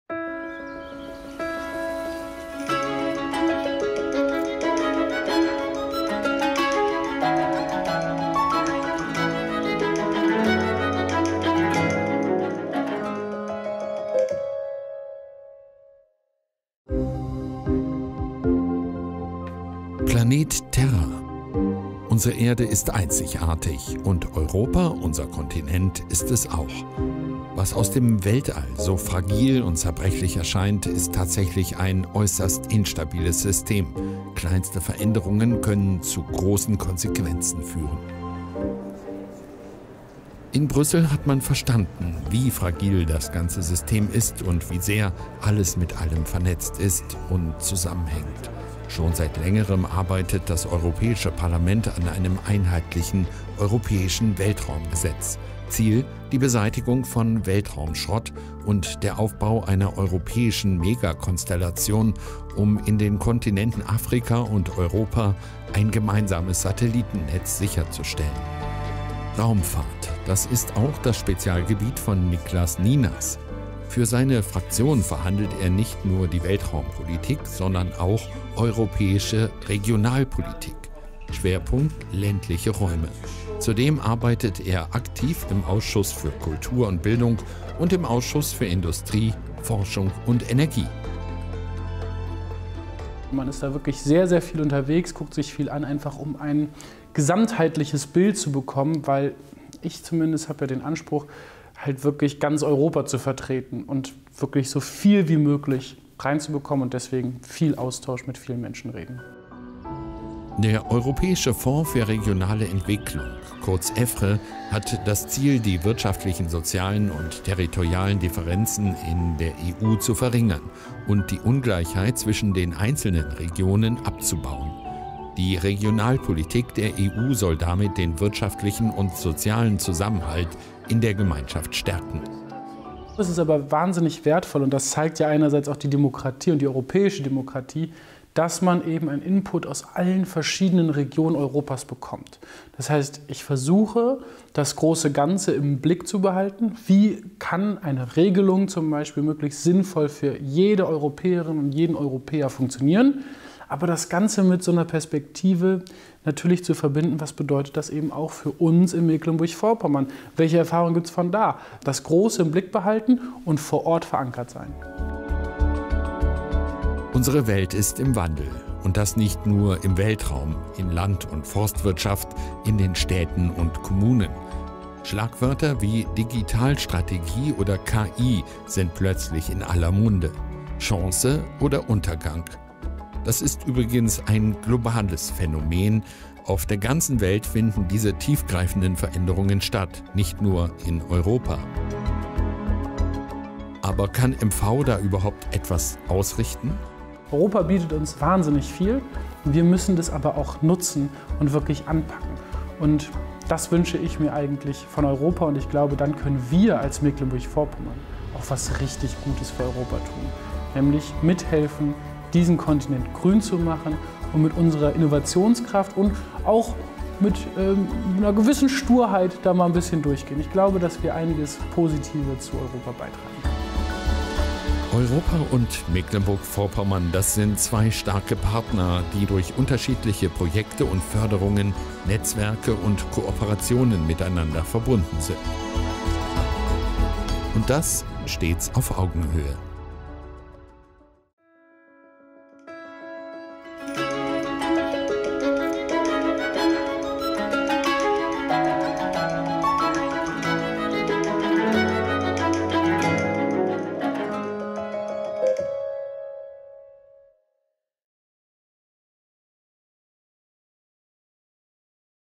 Planet Terra. Unsere Erde ist einzigartig. Und Europa, unser Kontinent – ist es auch.
Was aus dem Weltall so fragil und zerbrechlich erscheint, ist tatsächlich ein äußerst instabiles System. Kleinste Veränderungen können zu großen Konsequenzen führen. Für noch mehr spannende Themen und Videos besuchen gerne unsere